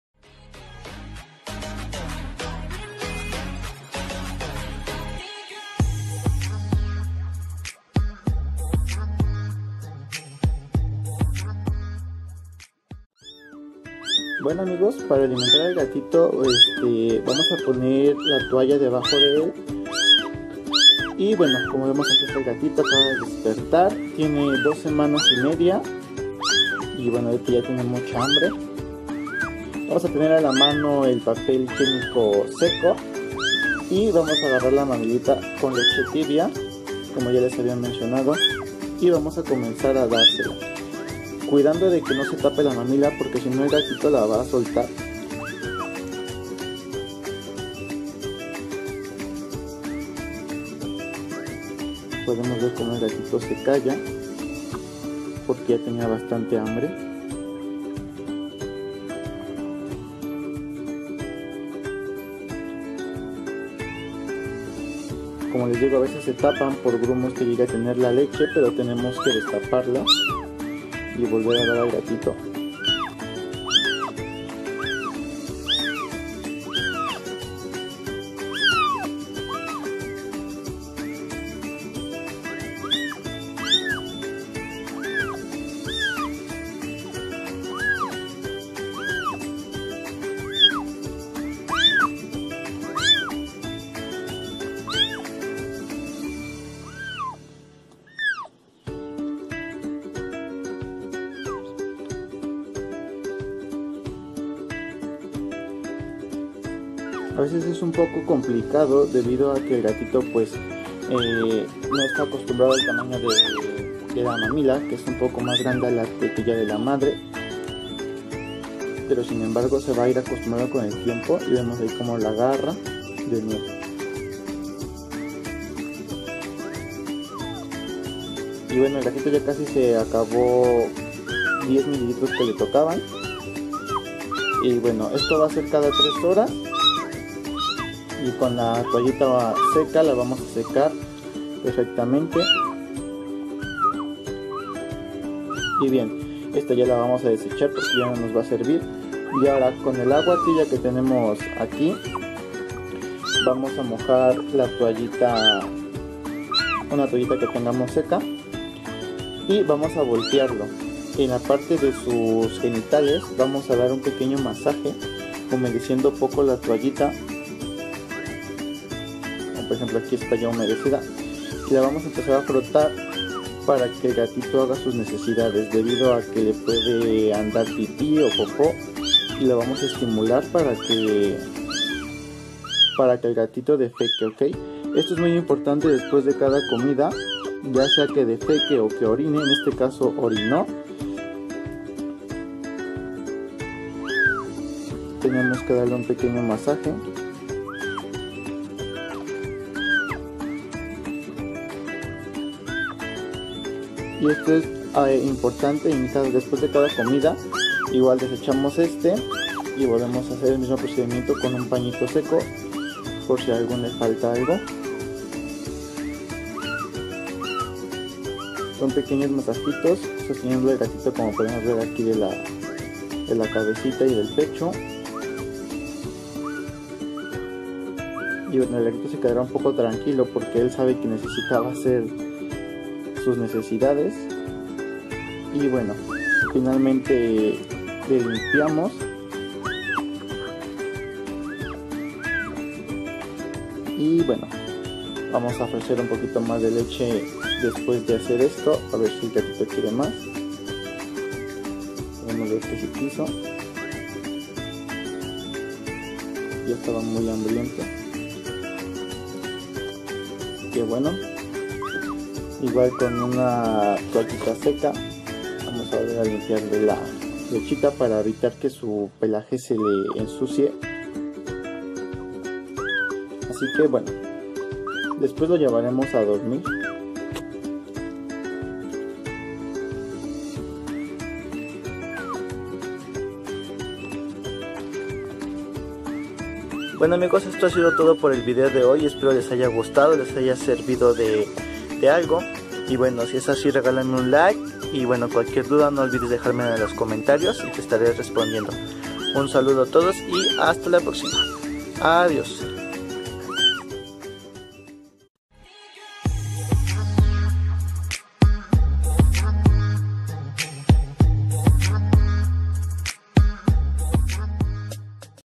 ¿Cómo alimentar a un gatito bebé?. |PLAN MASCOTA.
Amigos bienvenidos a un nuevo video en esta ocasión les muestro Cómo alimentar a nuestro gatito bebé si le falta su mamá, Espero te sirva y me regales un like un saludo a todos.